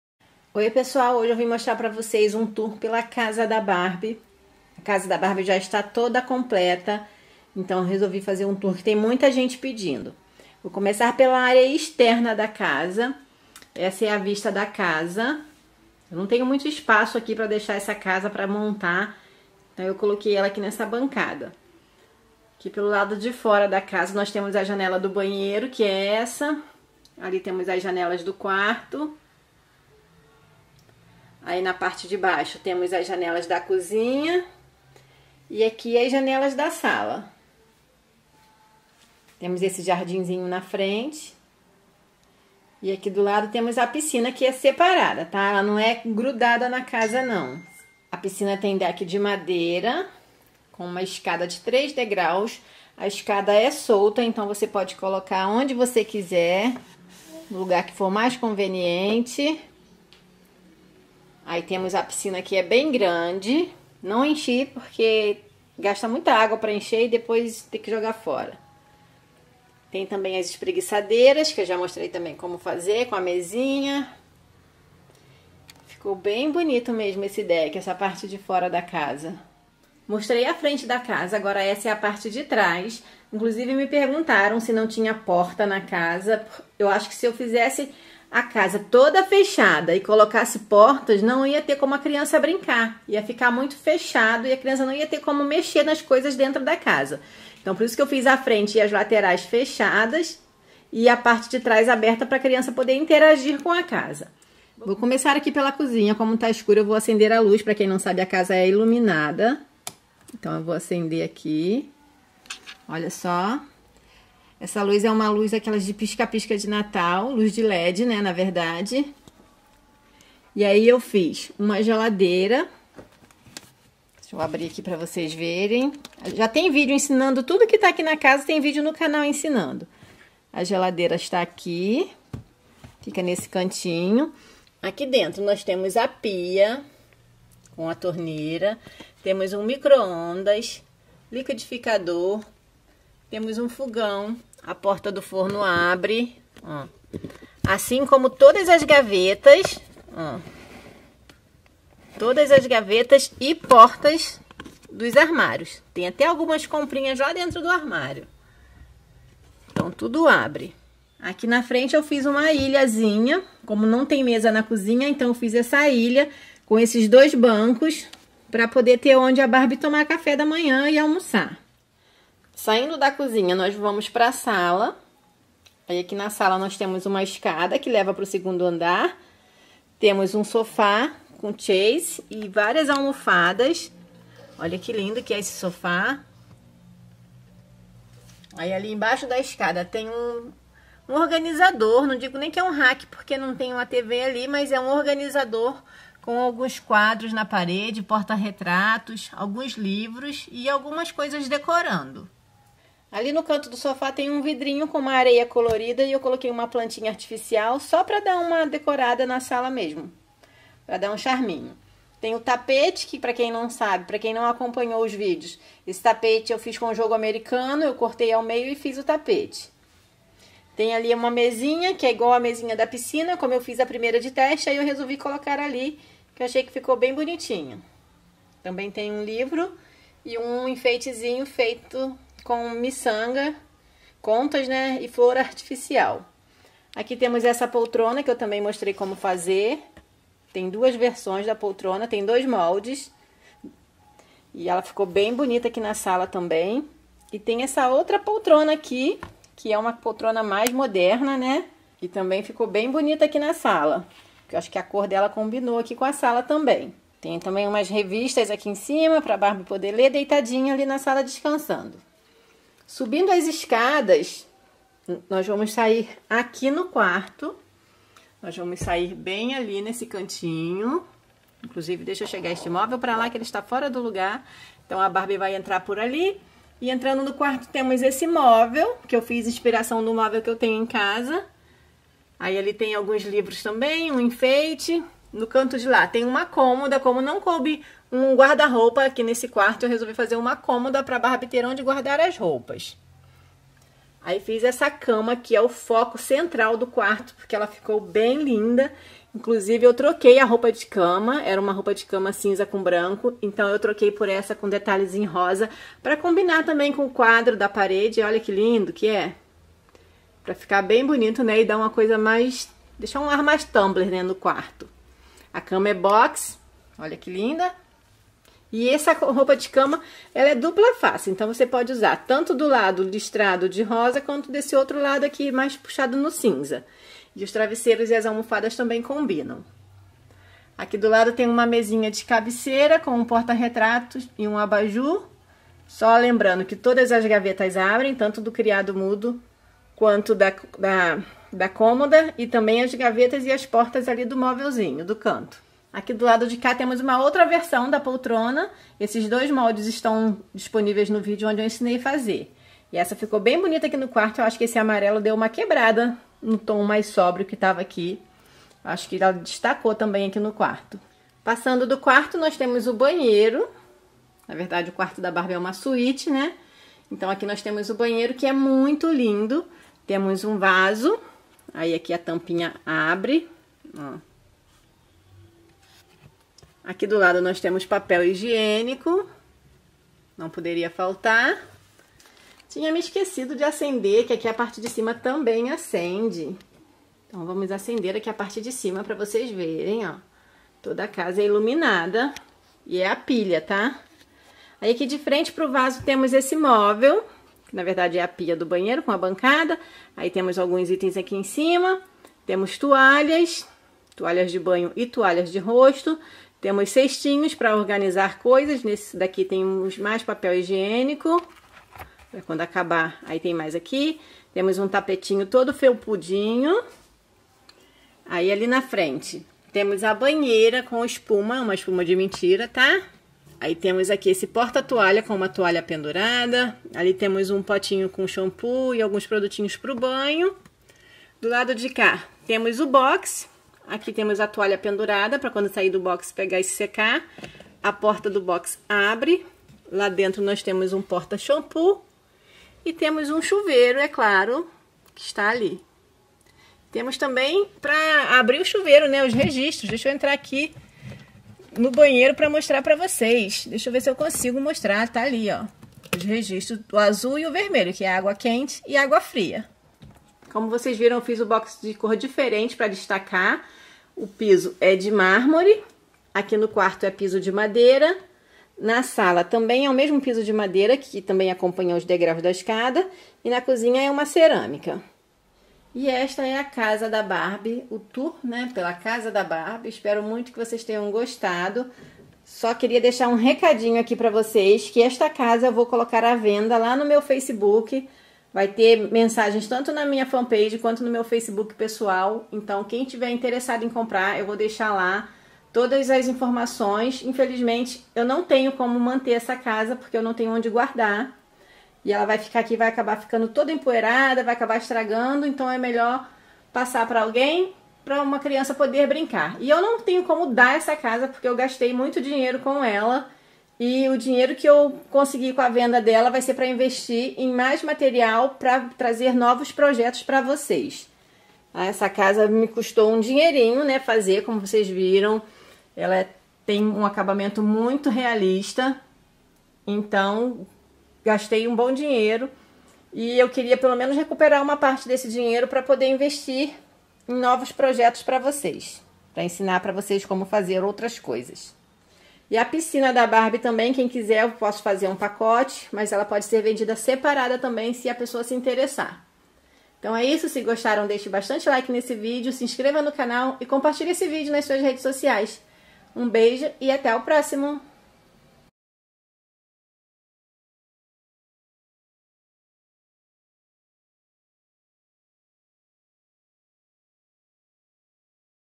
TOUR PELA CASA DUPLEX COM PISCINA DA BARBIE
Oi pessoal, vocês pediram e vim fazer um tour pela casa da Barbie.

● Glaucia Sioli
Caixa Postal: 93912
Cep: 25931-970
Rio de Janeiro
♡ Equipamentos ♡
● Câmera: Cânon G7x Mark II e Cânon T6i
● Editor: iMovie